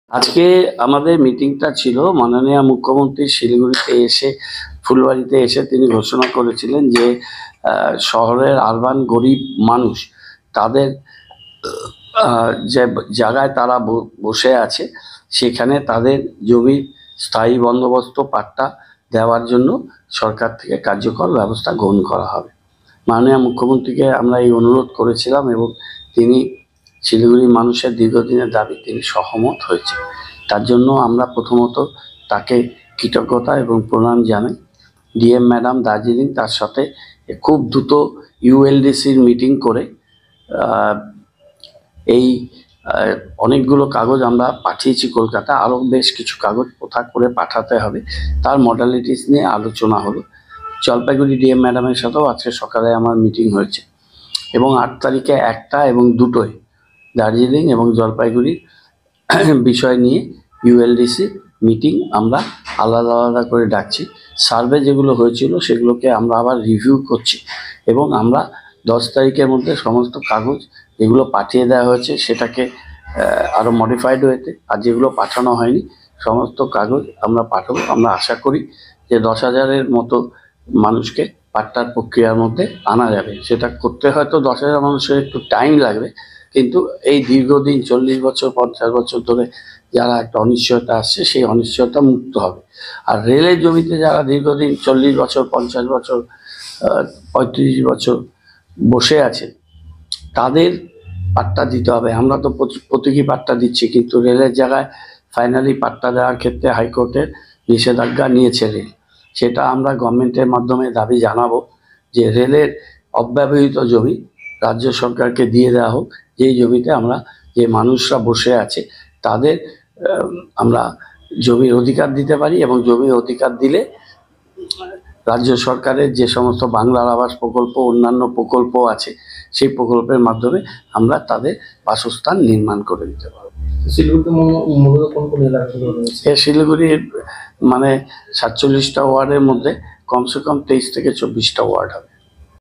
After the instructions of the CM, SMC begins the work of giving lease to slum dwellers.
After the instructions of the Chief Minister, Siliguri Municipal Corporation begins  the work of giving lease to slum dwellers.